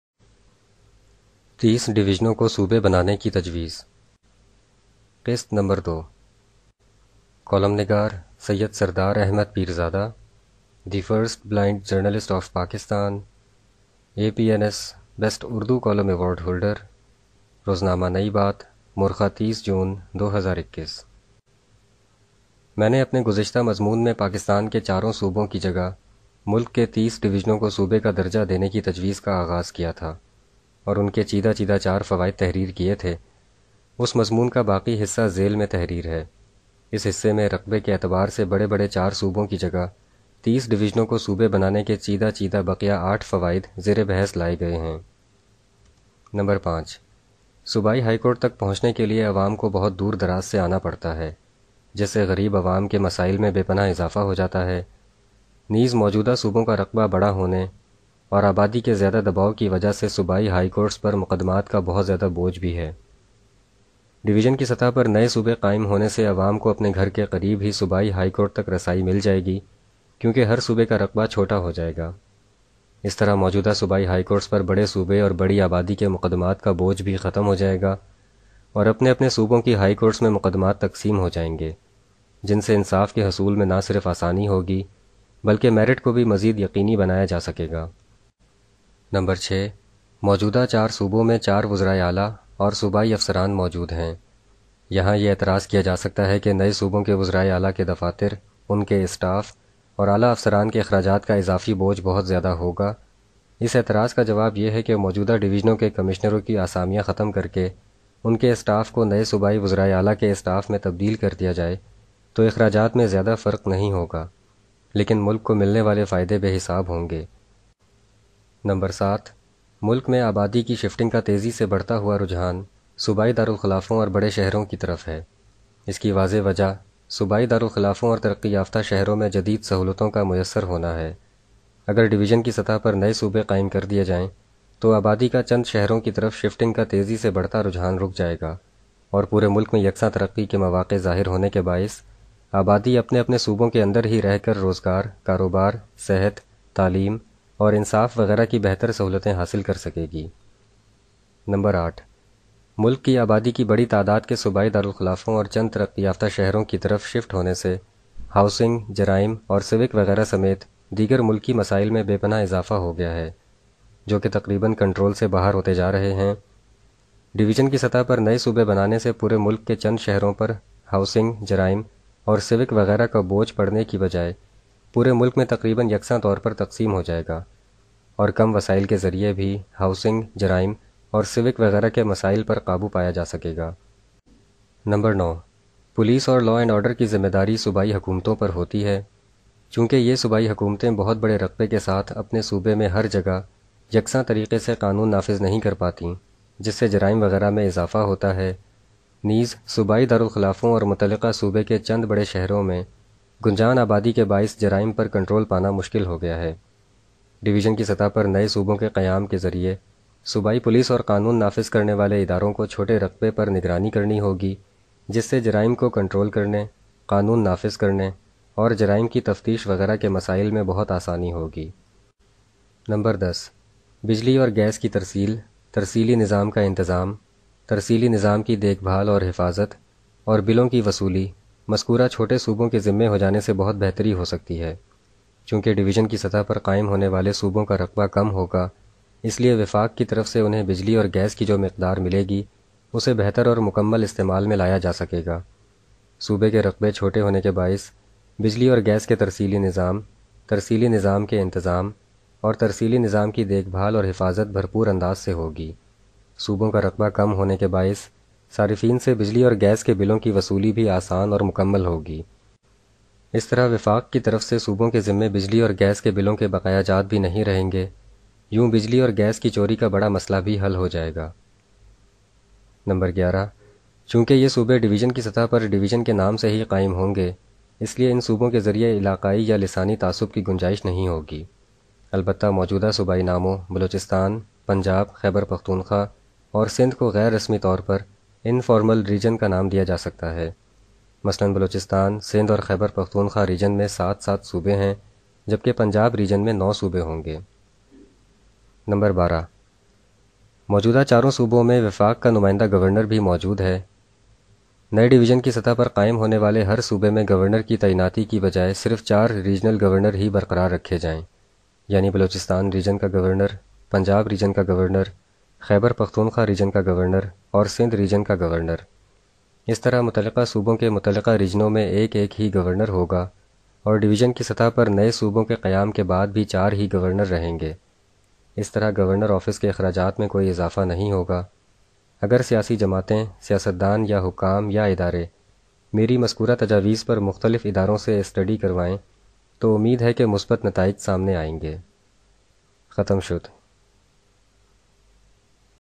Audio Urdu Column “تیس ڈویژنوں کو صوبے بنانے کی تجویز۔ قسط2”
Audio Urdu Column “تیس ڈویژنوں کو صوبے بنانے کی تجویز۔ قسط2” by Syed Sardar Ahmed Pirzada
The First Blind Journalist of Pakistan
Daily Nai Baat, 30th June, 2021